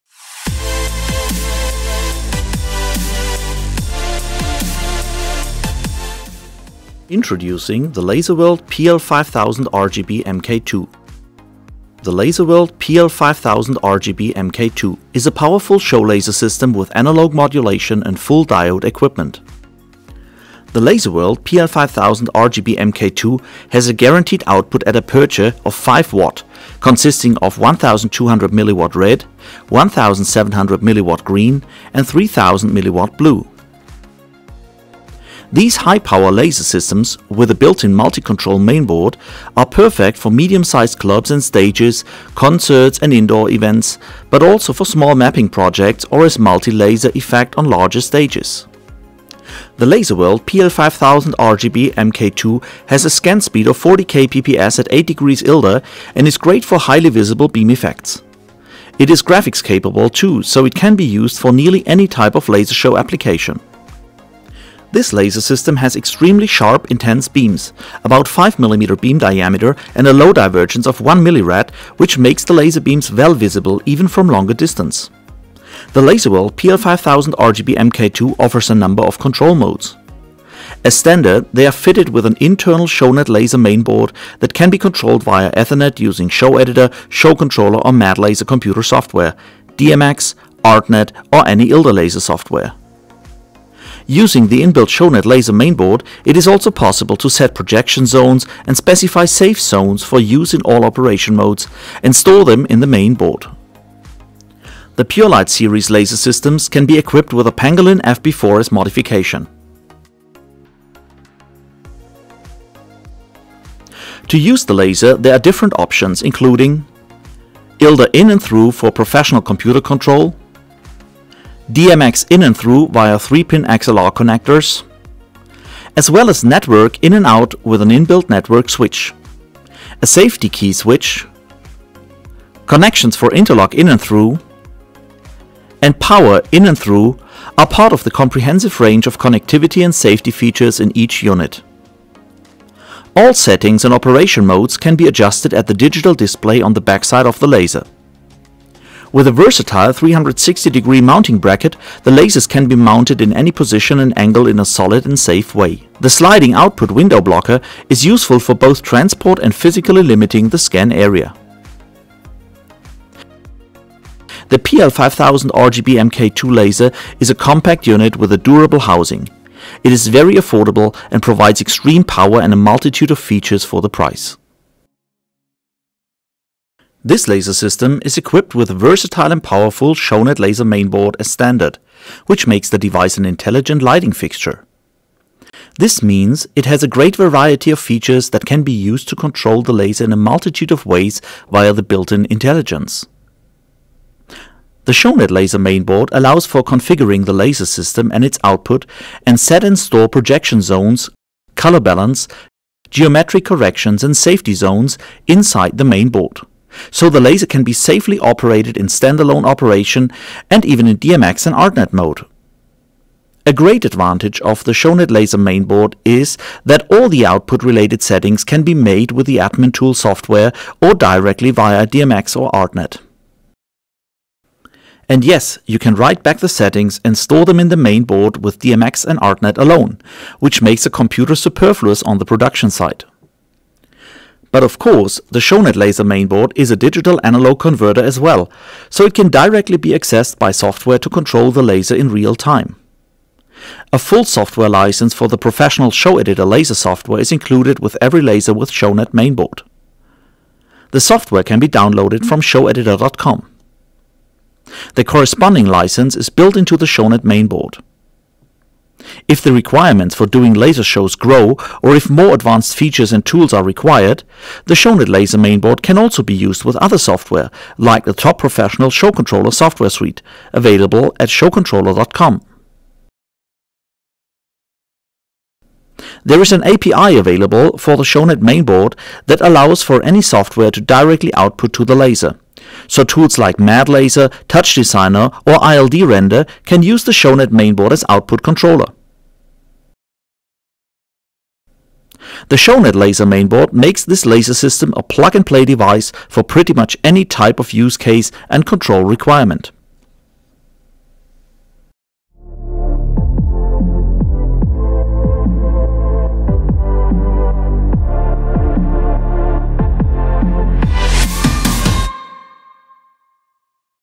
Laserworld PL-5000RGB MK2 product video
The Laserworld PL-5000RGB MK2 is a high power, full color laser with built in multi-control mainboard. Amazing DMX / ArtNET control with internal safety settings making it simple to control multiple units along with the rest of your DMX lighting. Full feature laser show software license included! Sealed optics section for low maintenance Perfect for large nightclub installs, indoor events, small outdoor events and look amazing on large productions in numbers running DMX chases. Including waterproof flightcase.

- 5'000 mW guaranteed power
- Graphics capable - 40kpps @ 8° ILDA
- Max scan angle 50°
- Full colour mixing - analog modulation
- Sharp intense beams – ca. 5.0 mm beam diameter and low divergence of 1.0 mrad
- Save safety settings direct to the laser and they apply in all modes
- Link multiple units with linking Power, DMX and ILDA
- Free computer control software – Showeditor - upgradable to Showcontroller
- Multiple control modes - Auto, DMX, Artnet and ILDA
- Incl. waterproof flightcase